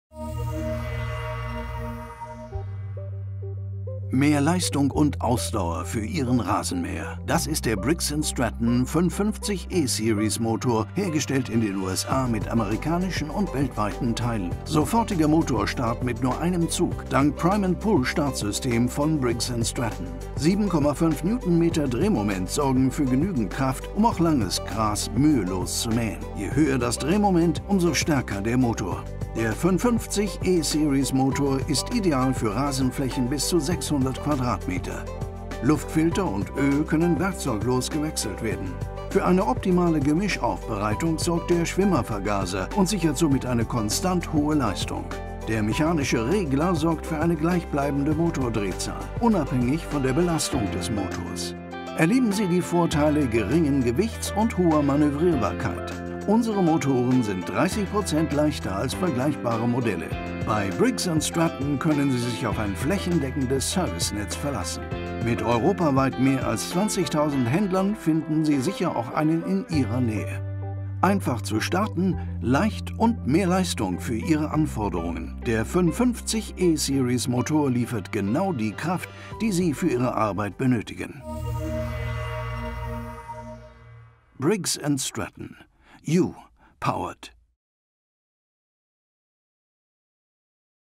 Der Briggs & Stratton 550E SERIES Motor
Mehr Leistung und Ausdauer für Ihren Rasenmäher. Er ist einfach zu Starten, leicht und mehr Leistung für Ihre Anforderungen.

Der 550E SERIES Motor liefert genau die Kraft, die Sie für Ihre Arbeit benötigen.